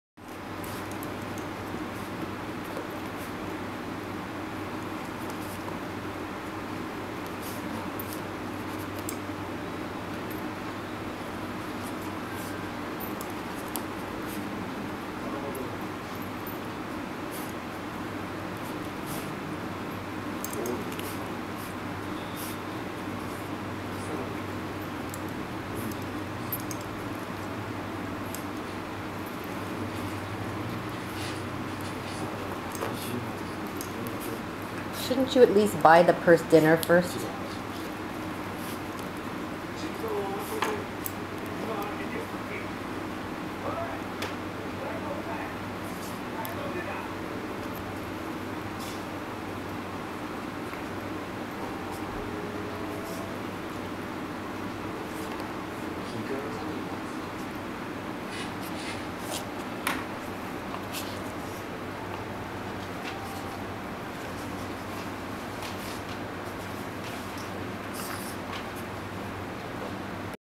Koa - Shih Tzu Maltese Cute Dog loves Bag
Koa loves the smell of leather, it's like catnip for him.  Here he is rolling on my new purse.